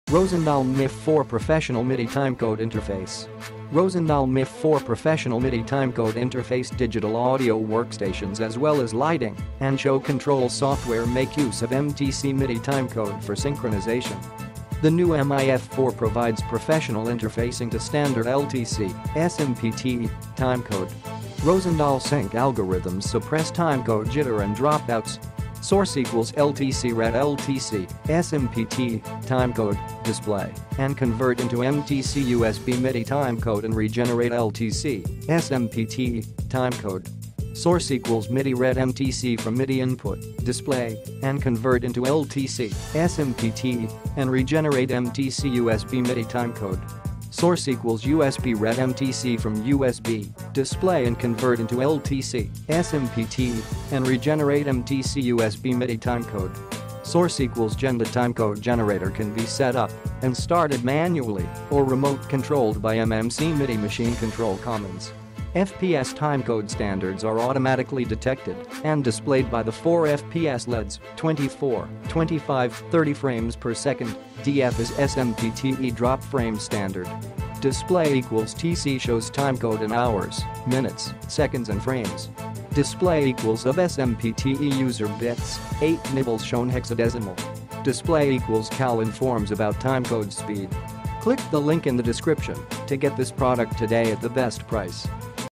Rosendahl MIF 4 Professional MIDI Timecode Interface - Overview
Rosendahl MIF 4 Professional MIDI Timecode Interface Digital audio workstations as well as lighting and show control software make use of MTC midi timecode for synchronisation. The new mif4 provides professional interfacing to standard LTC (SMPTE) timecode. Rosendahl sync algorithms suppress timecode jitter and drop outs. SOURCE = LTC read LTC (SMPTE) timecode, display and convert into MTC/USB midi time code and regenerate LTC (SMPTE) timecode. SOURCE = MIDI read MTC from MIDI input, display and convert into LTC (SMPTE) and regenerate MTC/USB midi timecode. SOURCE = USB read MTC from USB, display and convert into LTC (SMPTE) and regenerate MTC/USB midi timecode. SOURCE = GEN the timecode generator can be set up and started manually or remote controlled by MMC midi machine control comands. FPS timecode standards are automatically detected and displayed by the four FPS LEDs (24, 25, 30 Frames Per Second, DF is SMPTE Drop Frame standard). DISPLAY = TC shows timecode in hours, minutes, seconds and frames. DISPLAY = UB SMPTE user bits, 8 nibbles shown hexadezimal. DISPLAY = CAL informs about timecode speed. 29.97 non drop timecode for example is indicated by 30 FPS LED and calibration 0.9990. DISPLAY = REF PAL or NTSC video syncs (576i, 480i) as well as HD tri-level syncs (720p, 1080i, 1080p) are automatically detected and displayed along with their frame rate.